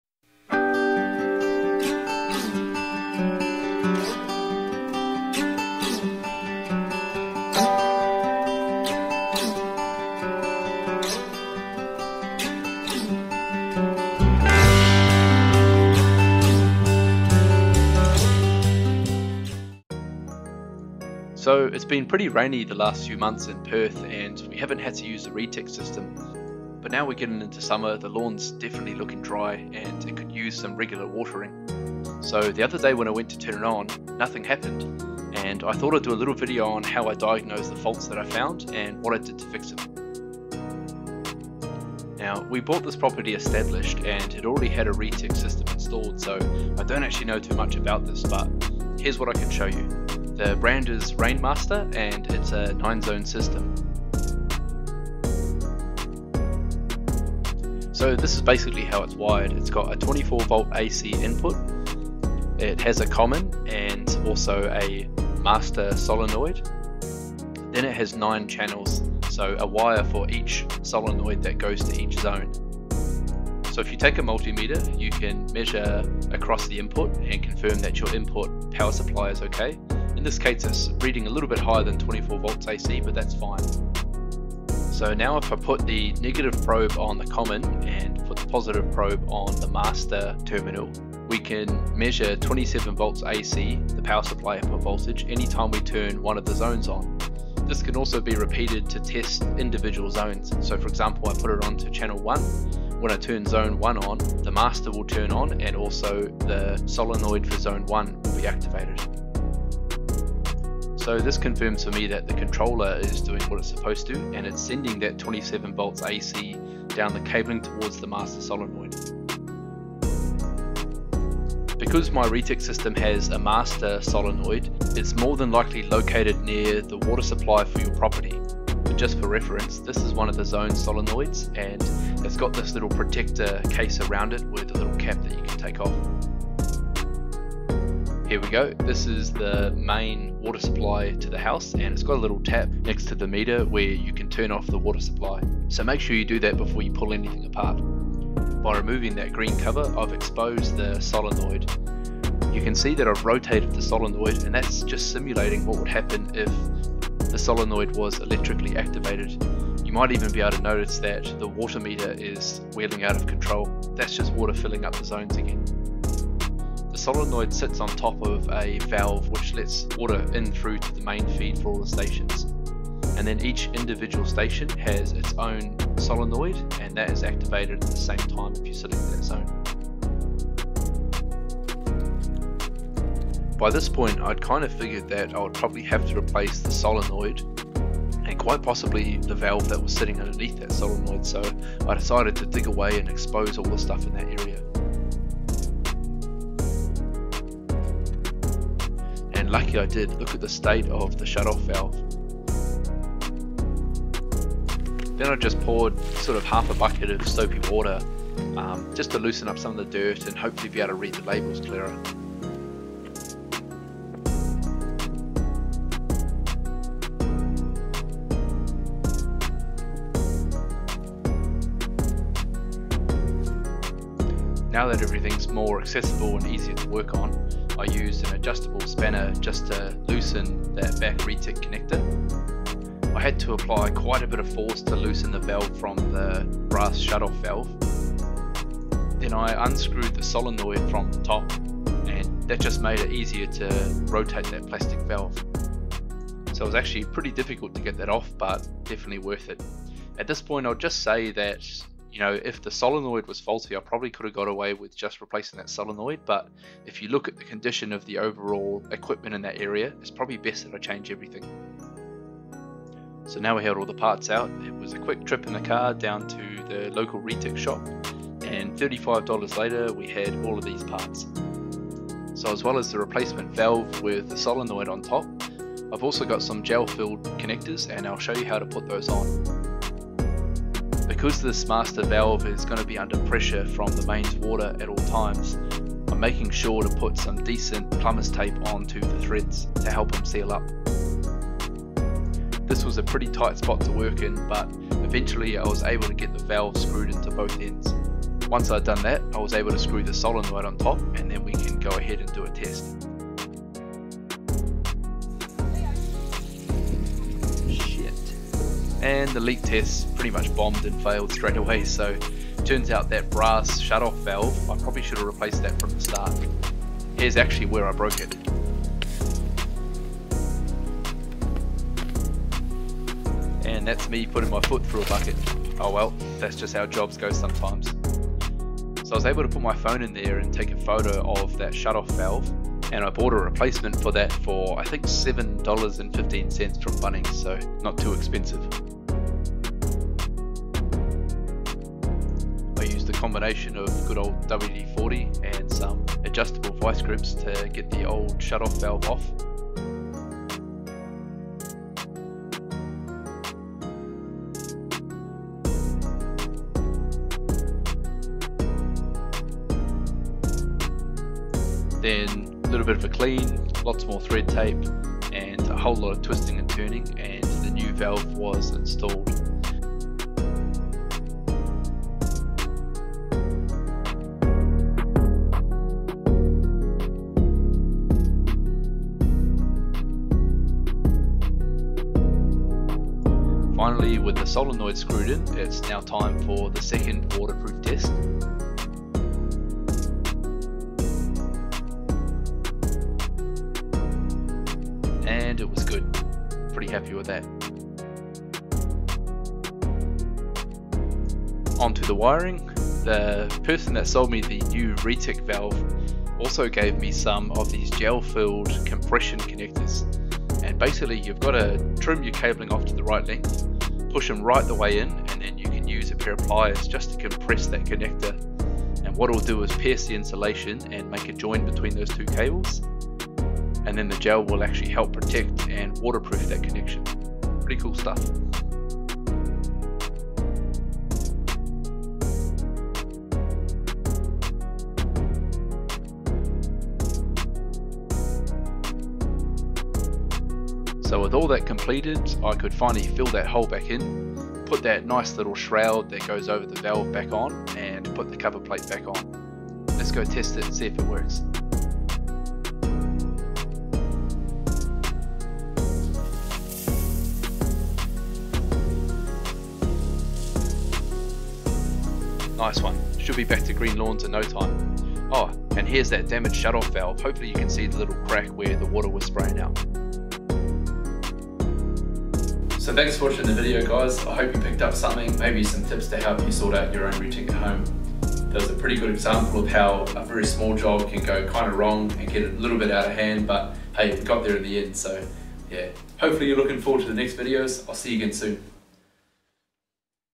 Reticulation Repair
Hey guys,

This video was unplanned, but gives us a good chance to look at how to diagnose a retic system. I show you how to use a multimeter to test if each solenoid is being activated, what to do if you find a broken one and how to replace it.